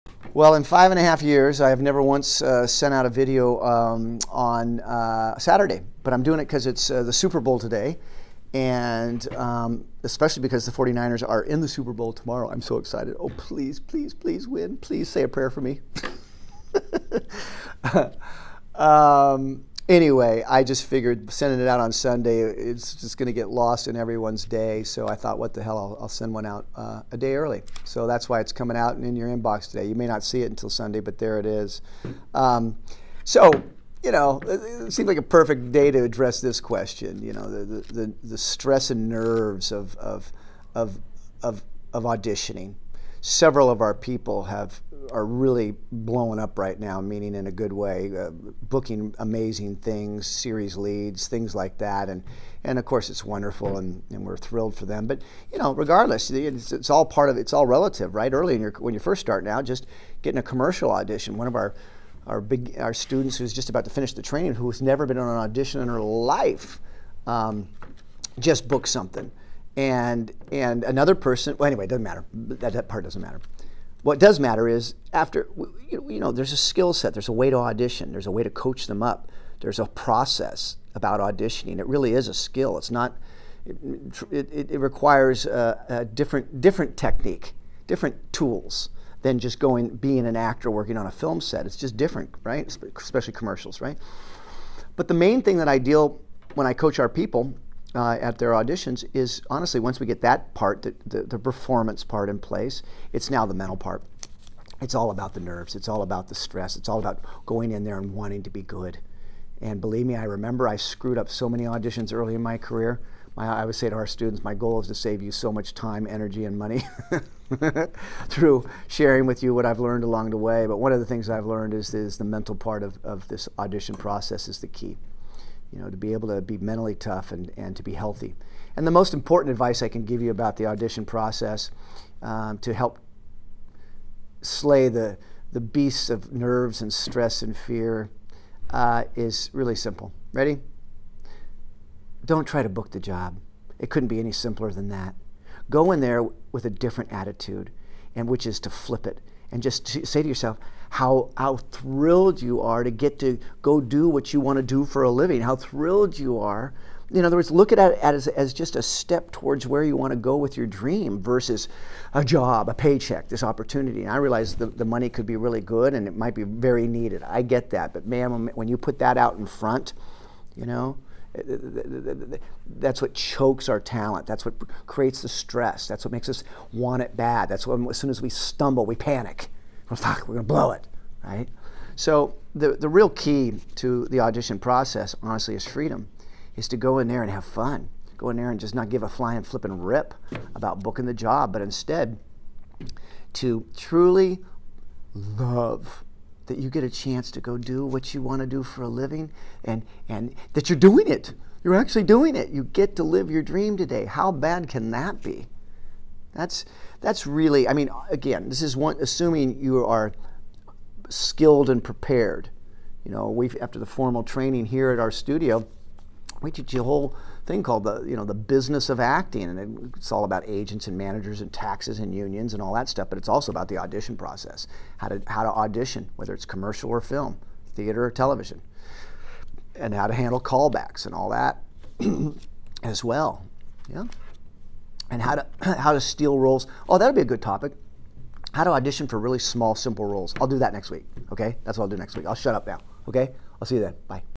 Advice From an Acting Teacher: How to get over nerves in auditions
There are keys to auditioning and one of THE biggest for you to address is nerves because I don't care how talented you are and how much natural ability you may be blessed with, if you aren't skilled at auditioning then it will be hard to build a career.

And one of THE most important aspects to auditioning is addressing your nerves. I hope this helps you.